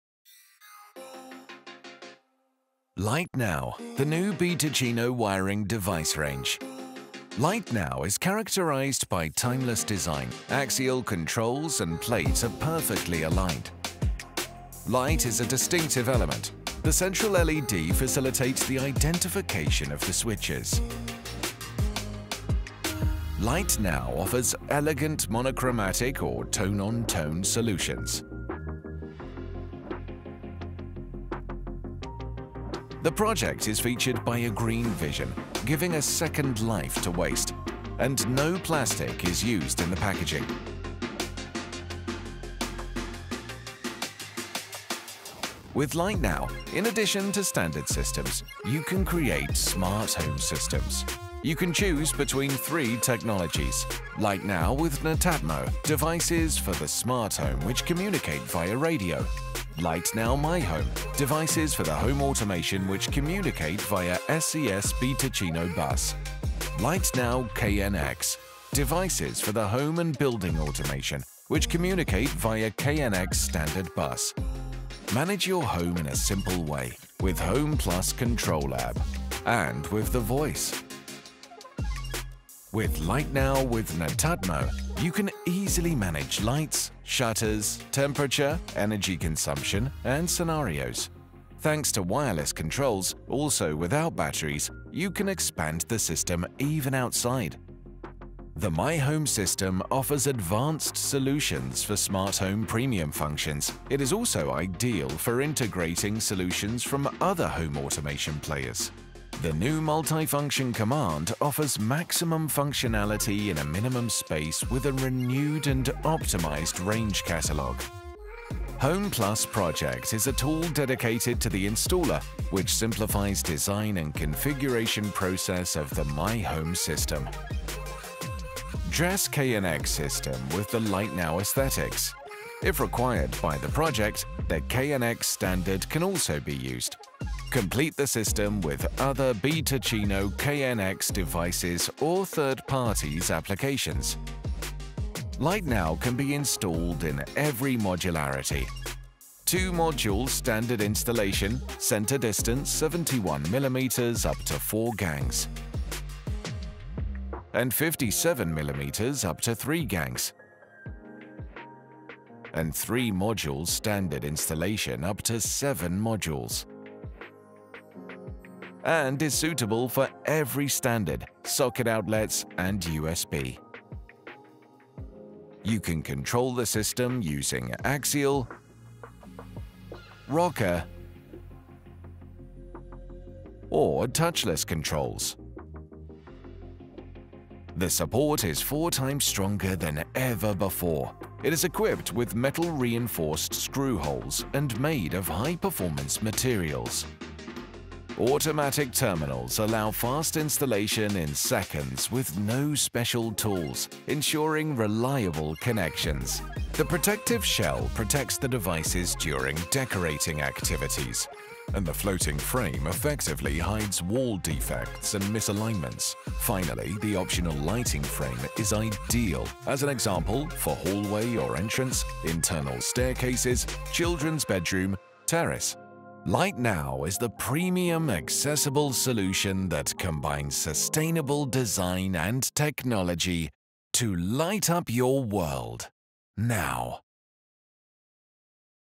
Light Now BTicino, beyond traditional systems: technology, high-quality installation, design
Light Now was designed to create a versatile, functional, easy, smart and sustainable program:
It is characterized by:
• standard and smart installations (three technologies: with Netatmo, MyHOME, KNX)
• cutting-edge solutions for high-quality installations
• green vision and sustainability, respecting the environment.
• design: three finishes for the mechanisms, white, dark and stone and 9 cover plate finishes
It can be installed in residential and tertiary environments (commercial environments and offices)